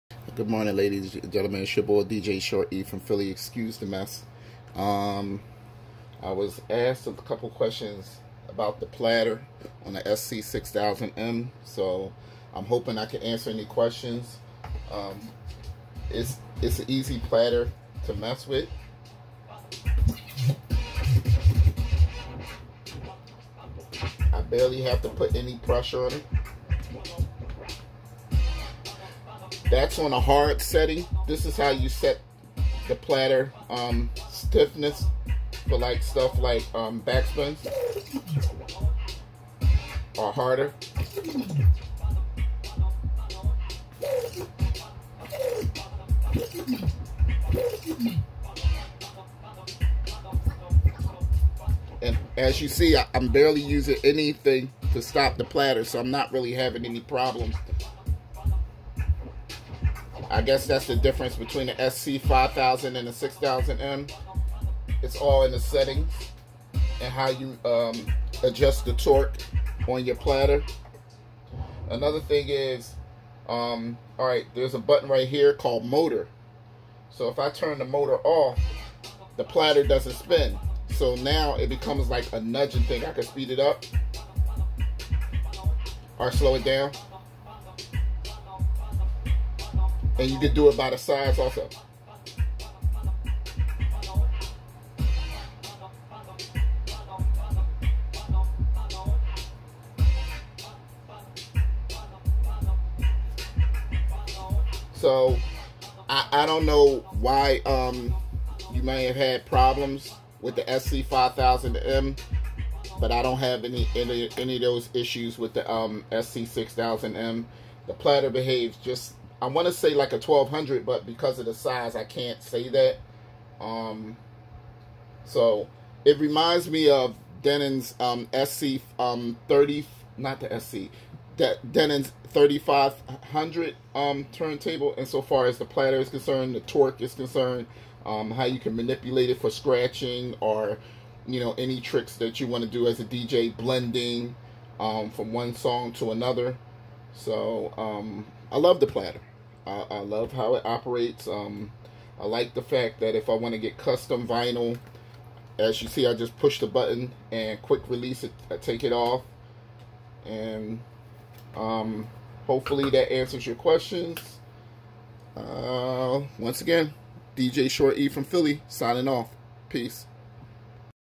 Denon dj sc6000m motorized platter information
I received a few questions about the platter performance on the denondj sc6000m in my video comments so, I felt compelled to make a video to answer... hope this helps.